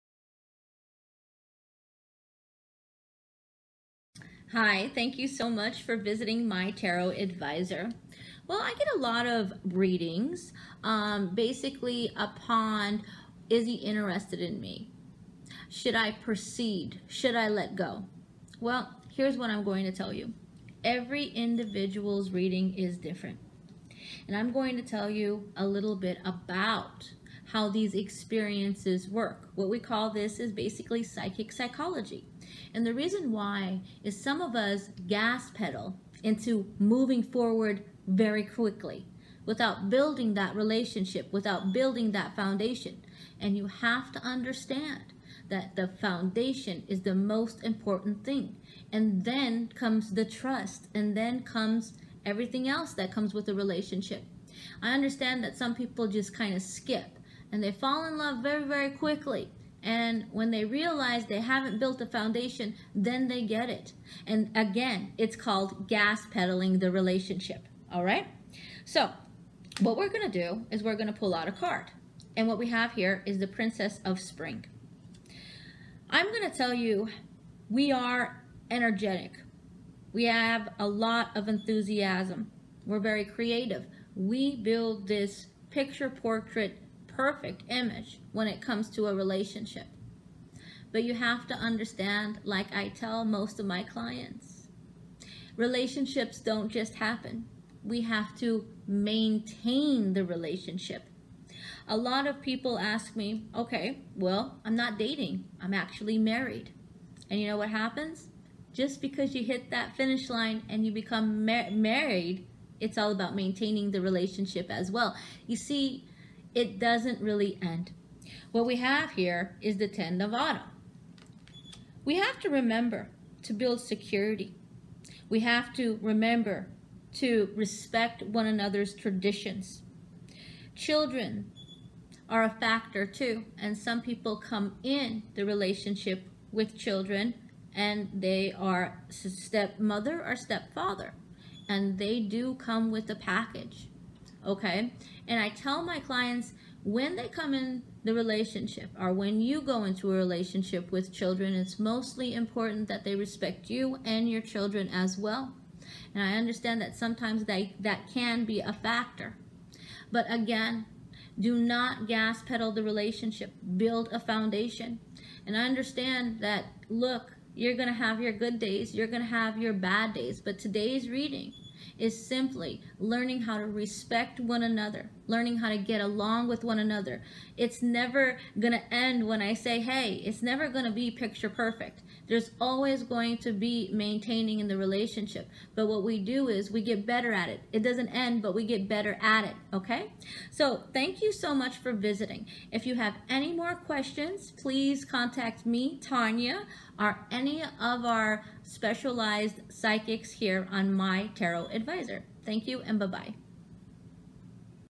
Every Reading Is Different
Today Tonya talks to us about How each reading is different, and how smilar questins can promote different responses!

You can speak with Tonya today at My Tarot Advisor and get 3 Free Minutes when you Join.